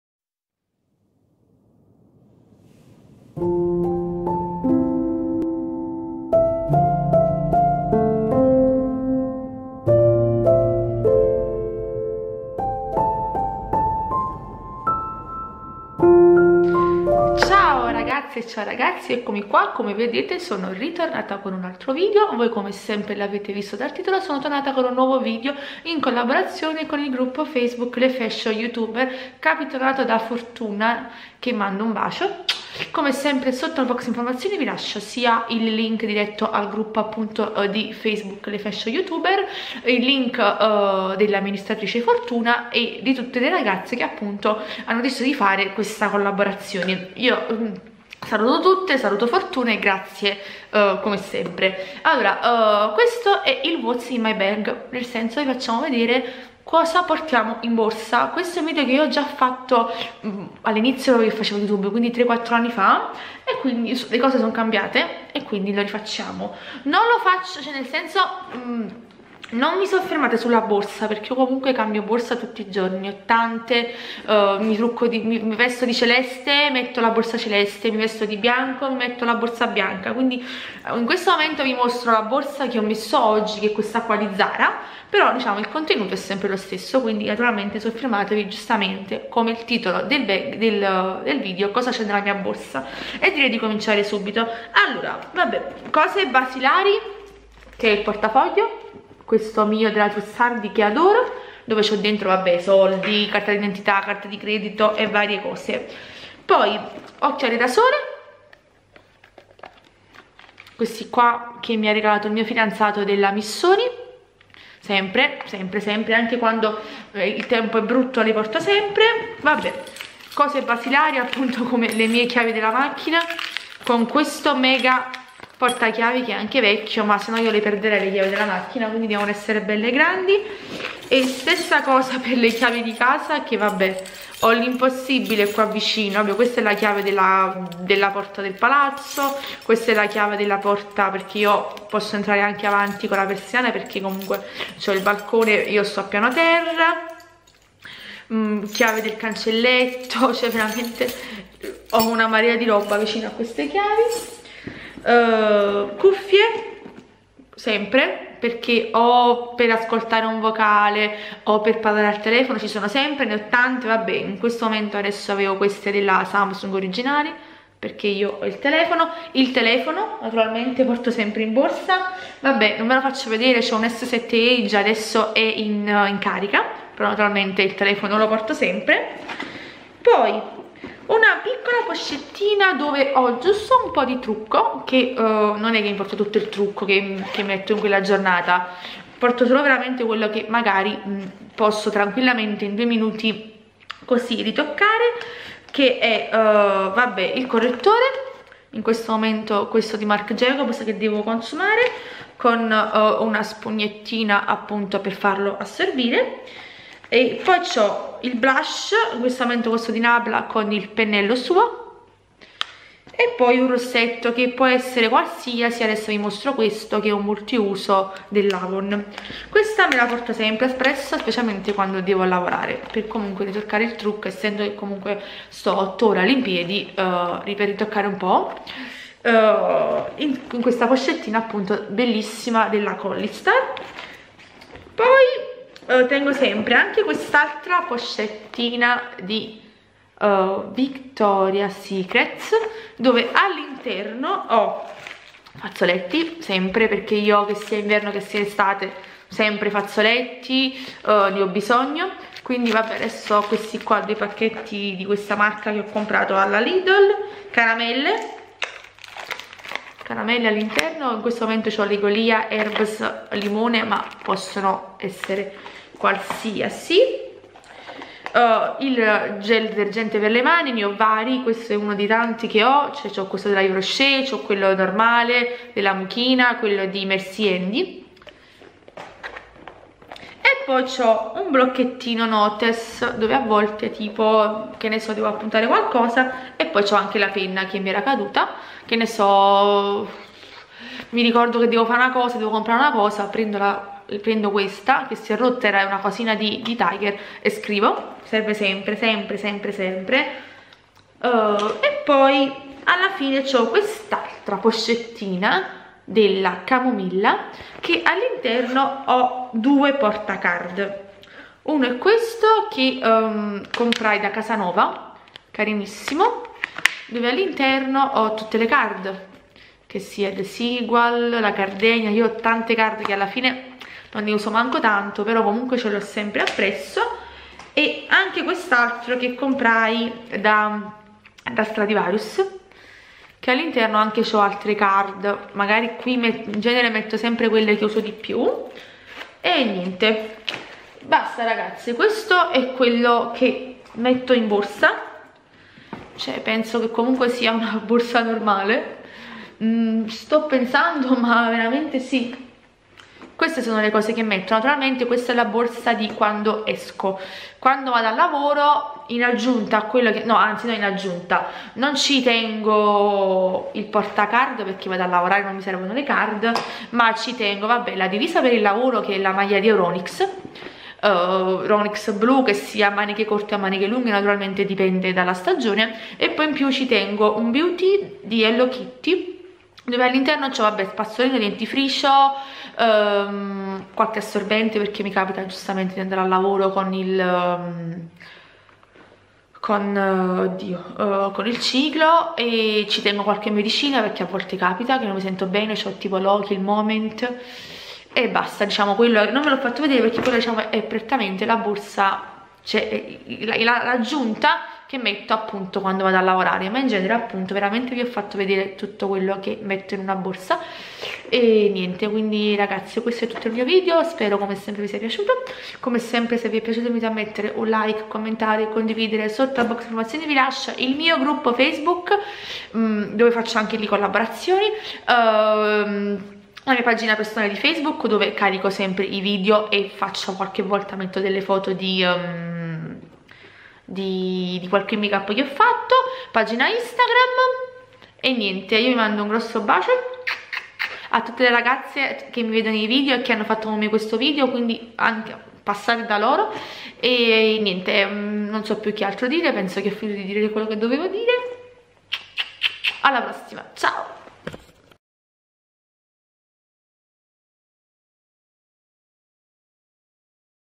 WHAT'S IN MY BAG FT LE FASHION YOUTUBERS
ECCOMI RITORNATA CON UN NUOVO VIDEO,UNA COLLABORAZIONE CON LE FASHIONIST GIRL,SPERO VI PIACCIA.

Canali partecipanti
Amministratrice